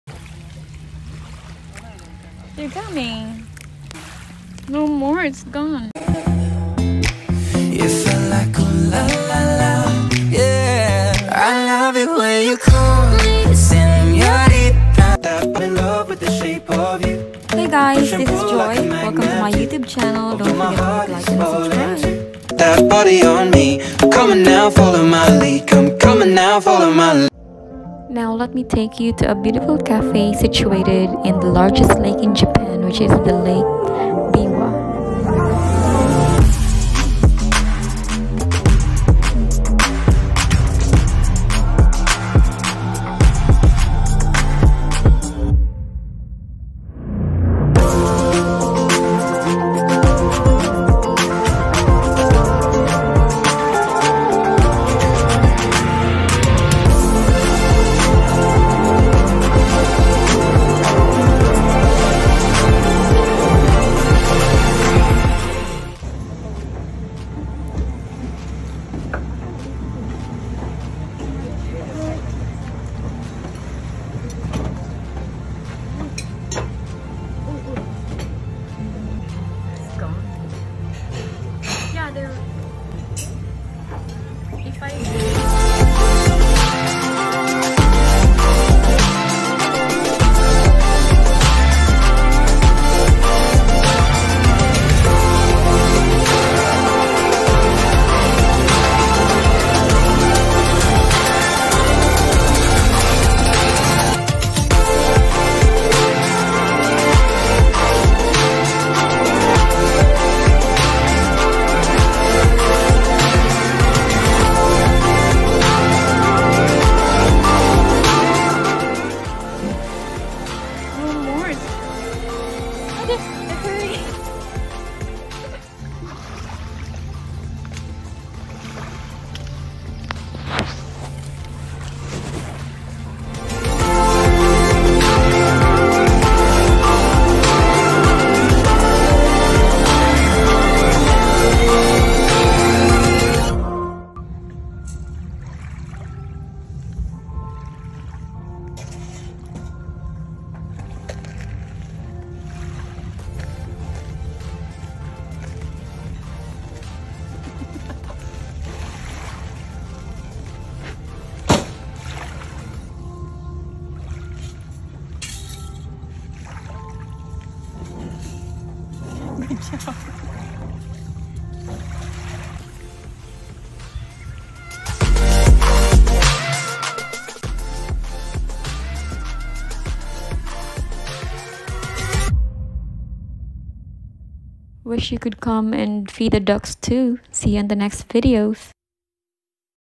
The Chalet in Omihachan Shiga where you can feed the ducks while you enjoy a beautiful lakeview 🇯🇵
The Chalet in Omihachan Shiga!
Where you can feed the ducks while you enjoy the beautiful view of Lake Biwa
A beautiful cafe’ overlooking Biwako the largest lake in Japan 🇯🇵